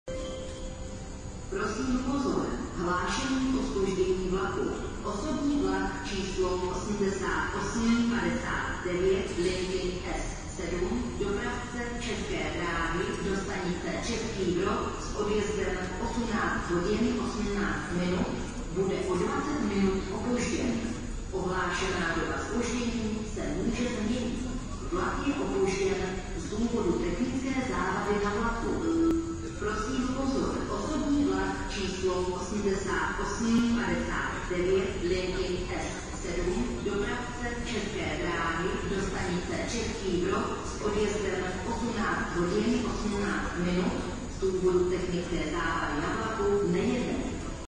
Hlášení Praha hlavní nádraží - Zpoždění a zrušení vlaku Os 8859, 29.8.2023 [INISS 2]
0:00 Os 8859 linky S7 ( bude o 20 minut opožděn )
0:29 Os 8859 linky S7 ( z důvodu technické závady na vlaku nejede )